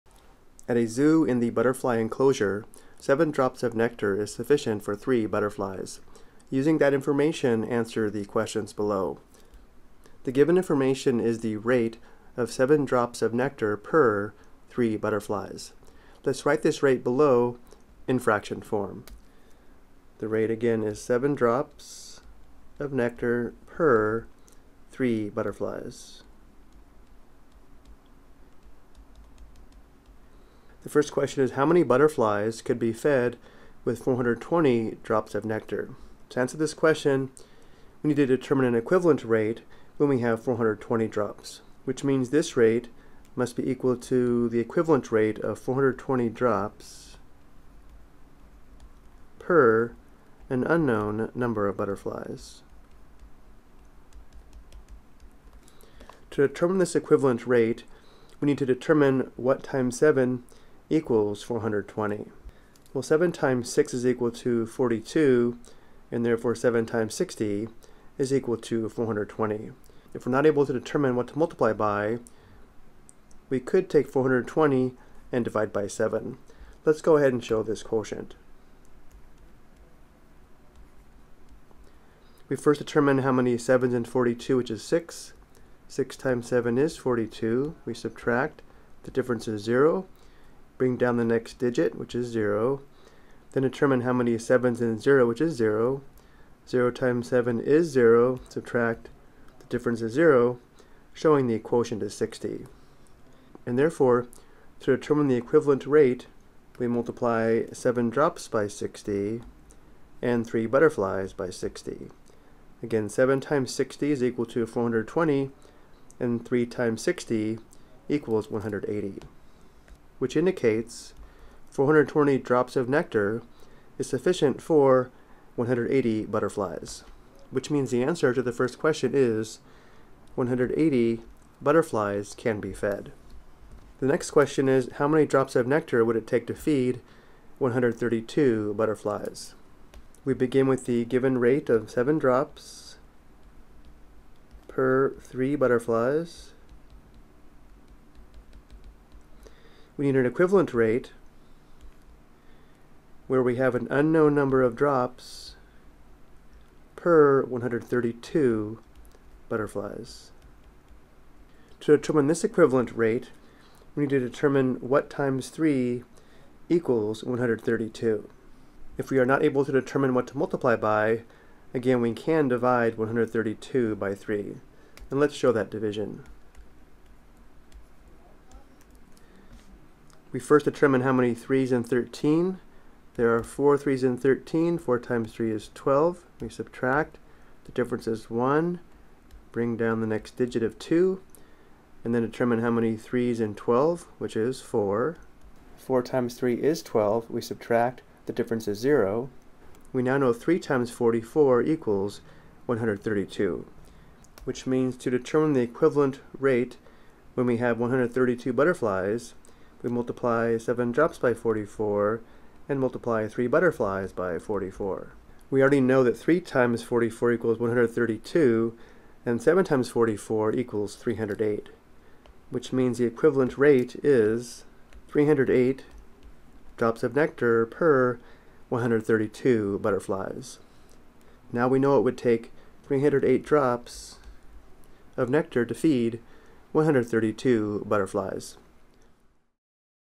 Proportion Application with No Cross Products
This video explains how to set up and solve basic proportions without using cross products.